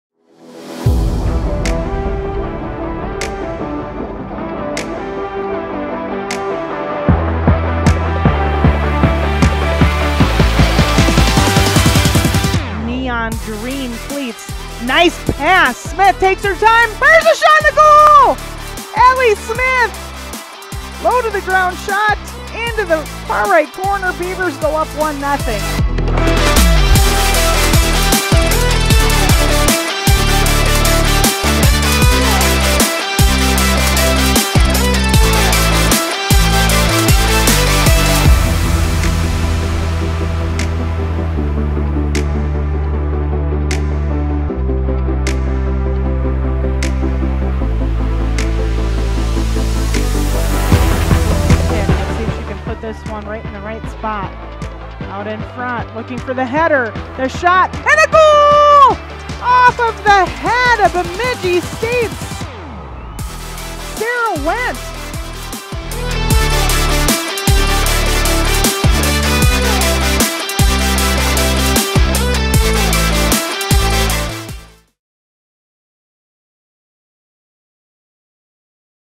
Bemidji State soccer highlights from NSIC Championship (11/14/21)
Watch the highlights from the Bemidji State soccer team's first appearance in the NSIC Tournament Championship match. The No. 6 Beavers fell to Minnesota State 4-2 at Chet Anderson Stadium.